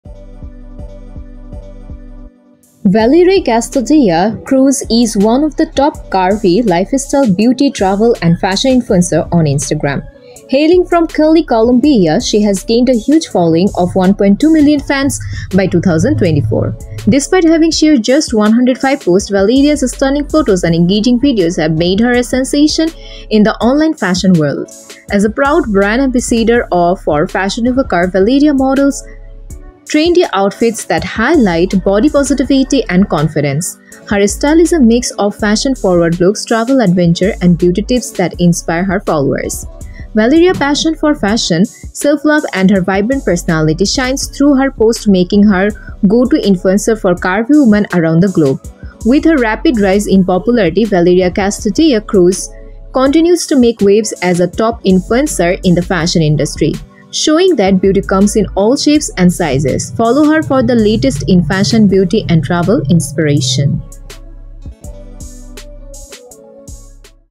Valeria Castañeda | Curvy Plus Size Model, Body Positivity, Biography, Wiki, & Facts
Valeria Castañeda Cruz is one of the top curvy lifestyle, beauty, travel, and fashion influencers on Instagram. Hailing from Cali, Colombia 🇨🇴, she has gained a huge following of 1.2 million fans by 2024. Despite having shared just 105 posts, Valeria's stunning photos and engaging videos have made her a sensation in the online fashion world.